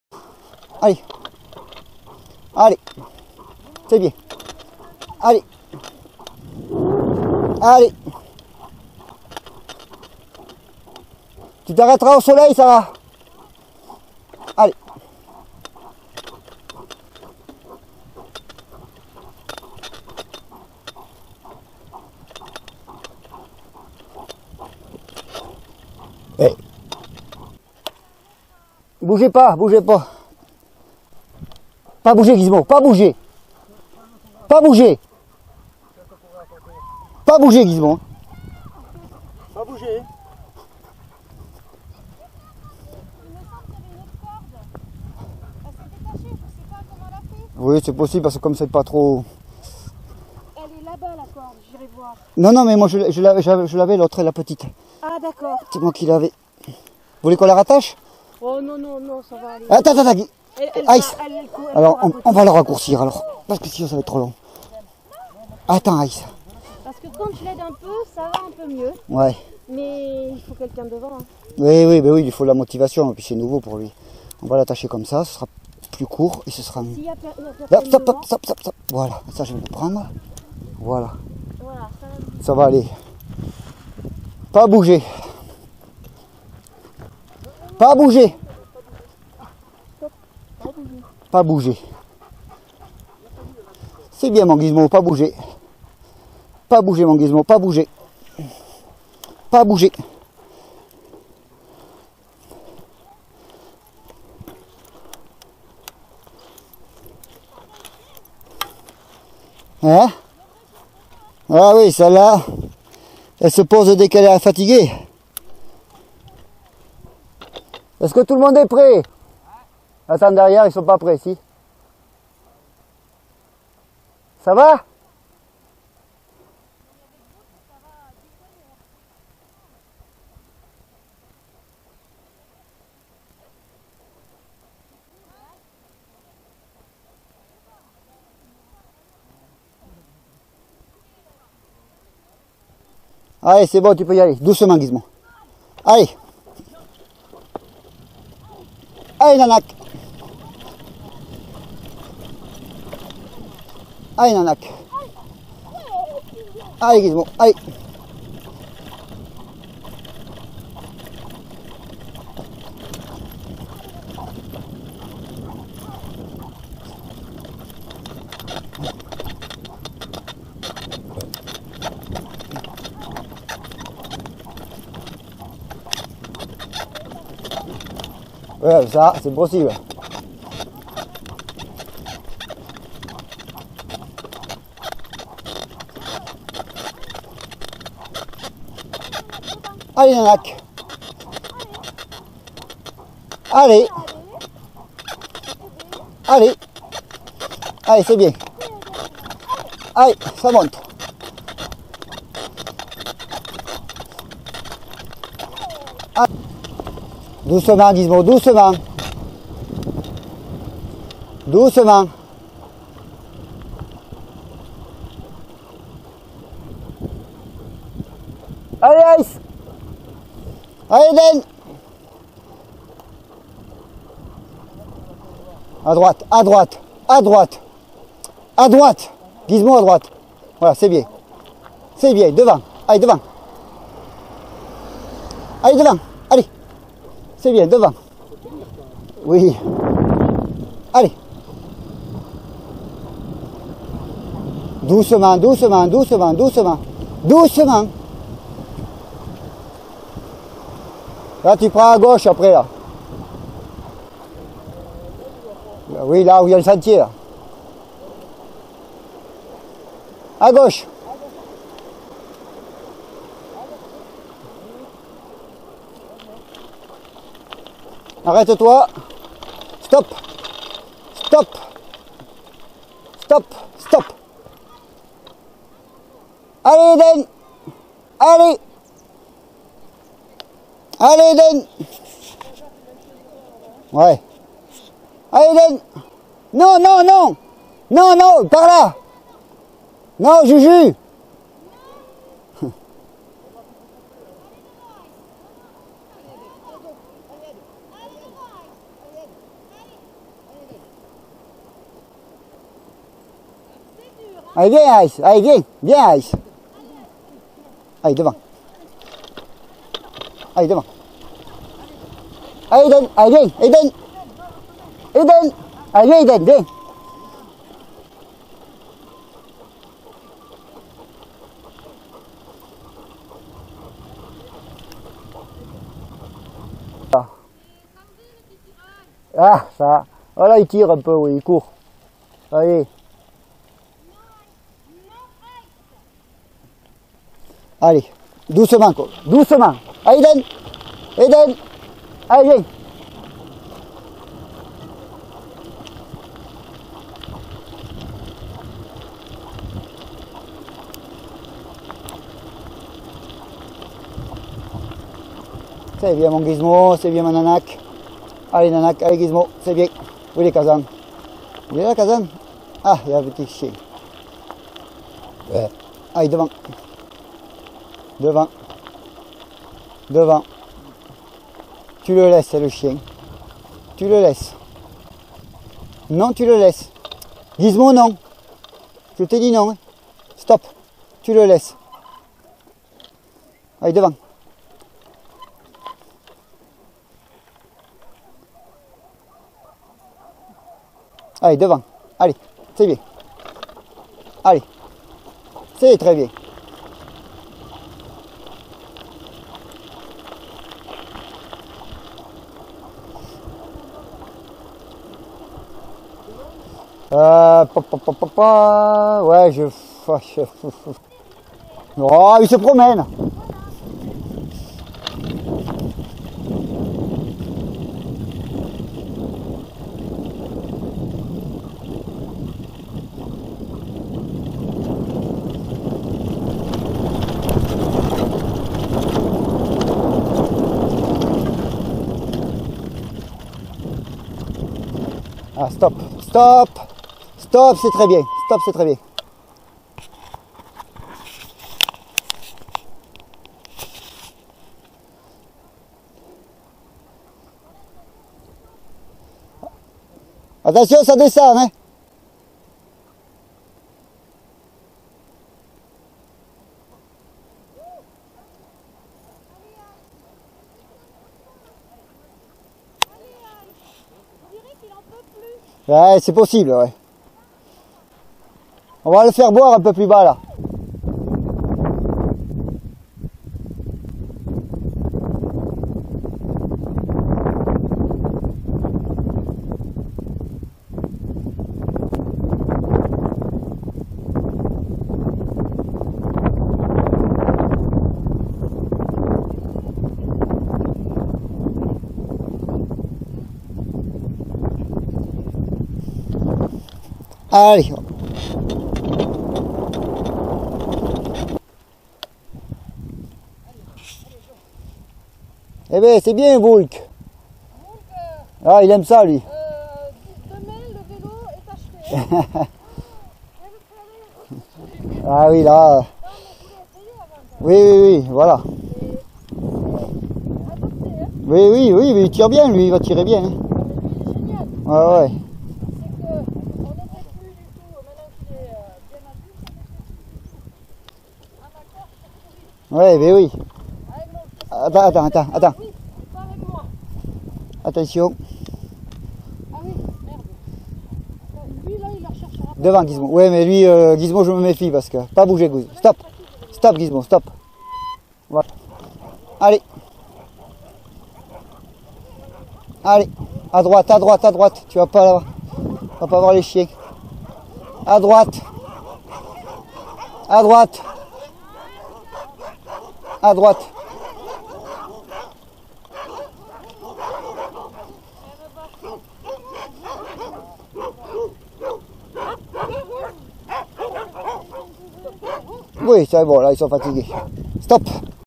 1er parcours cani vtt
1er parcours cani vtt De La Coleen d'Urok le Malamute d'Alaska Payolle chien de traineau